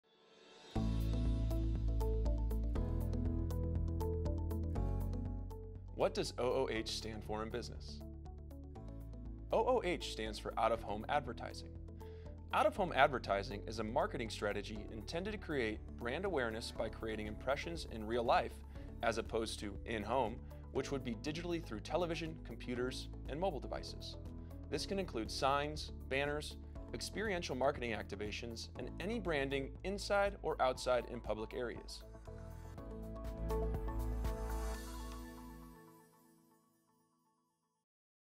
What Does OOH Stand for in Business?
OOH stands for Out Of Home Advertising. Out of home advertising is a marketing strategy intended to create brand awareness by creating impressions in real life as opposed to "in home" which would be digitally through television, computers and mobile devices. This can include signs, banners, experiential marketing activations and any branding inside or outside in public areas.